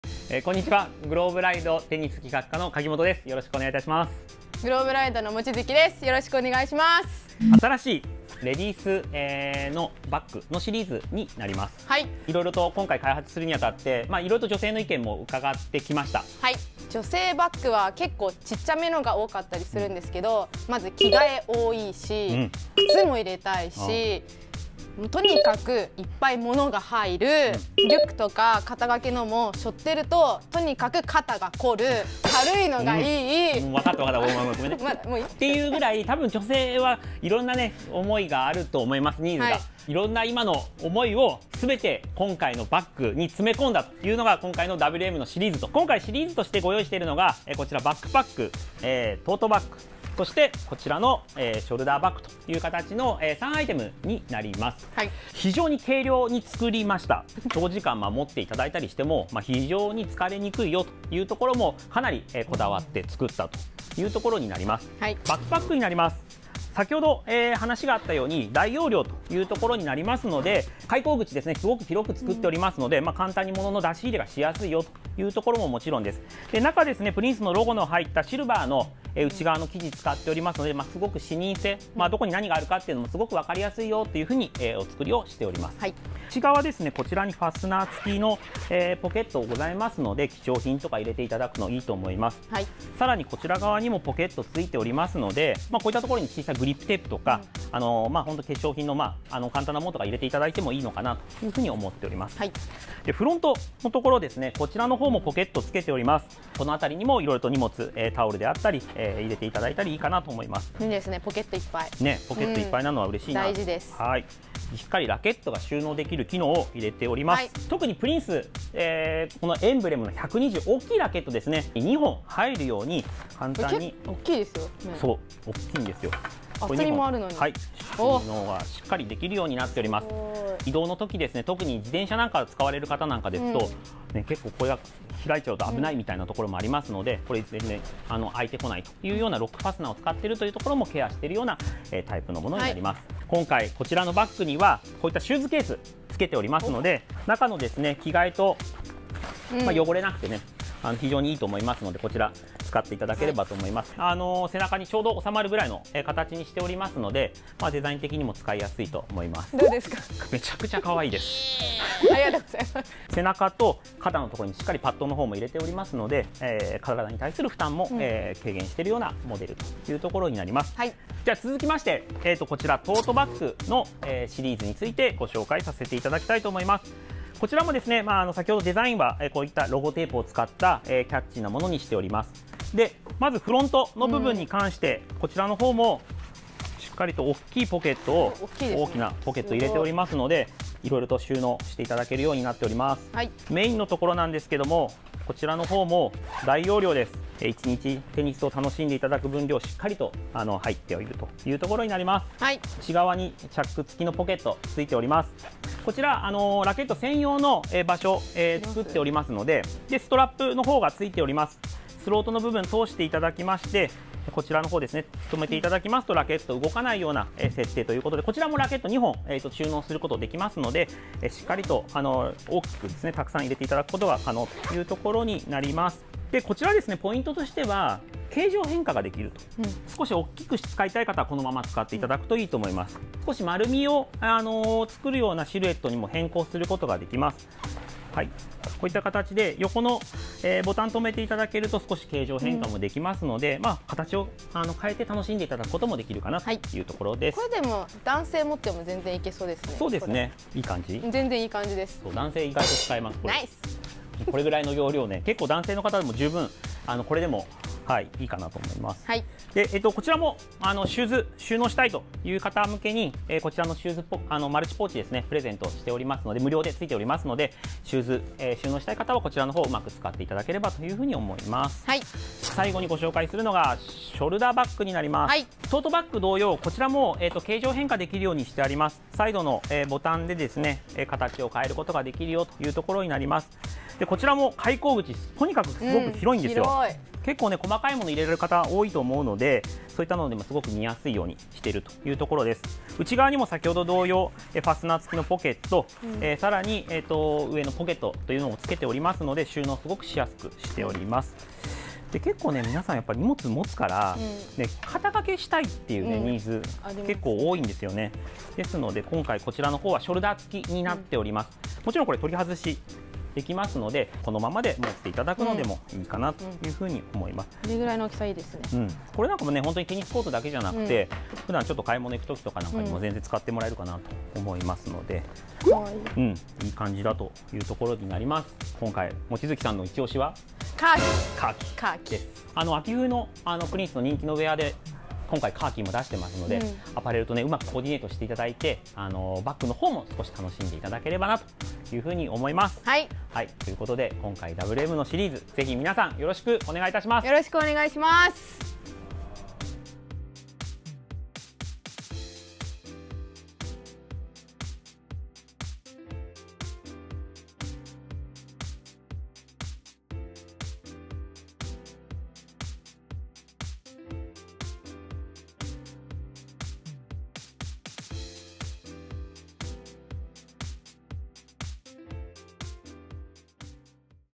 【プリンステニス】レディスプレーヤーにオススメ！！WMバッグシリーズ全貌公開！！
【プリンステニス】レディスプレーヤーからのリクエストを具現化したカジュアルテイストのテニスバッグ！！プリンスバッグ企画担当の柿本氏とモッチーからその全貌をご紹介します！！トートバッグは男性からも人気が出そうです！！
【プリンス】WM バッグシリーズ（2022年9月発売モデル）